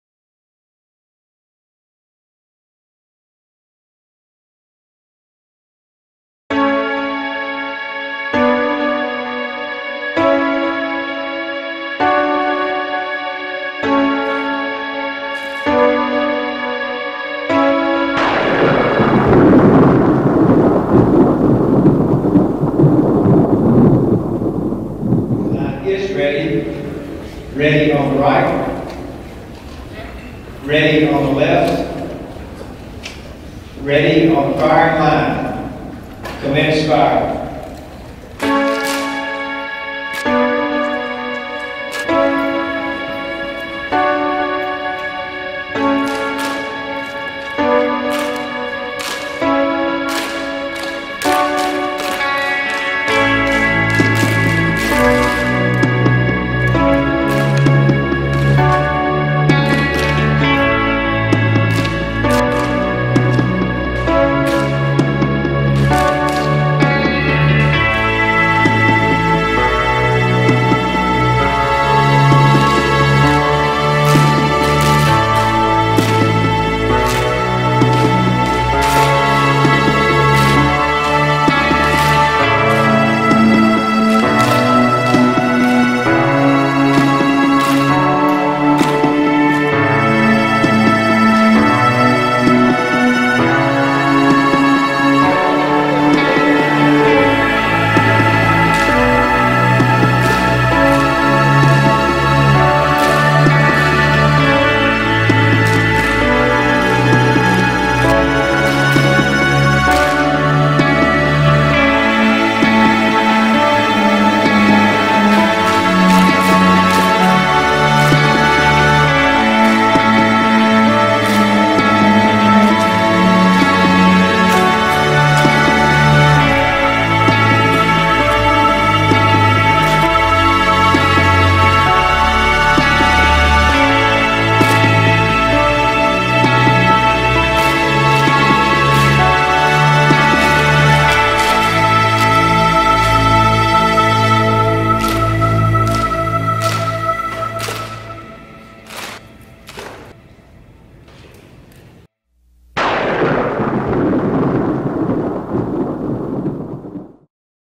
2017 Daisy National BB Gun Championships (4K)
The Pierre Junior Shooters took first place team honors at this year's event—the 52nd annual. Walton County 4-H claimed second, followed by Johnson County 4-H. Justin Merriman took individual gold. A record number of 71 teams qualified for the prestigious event, and here's just a  little of the action the shooters enjoyed after qualifying.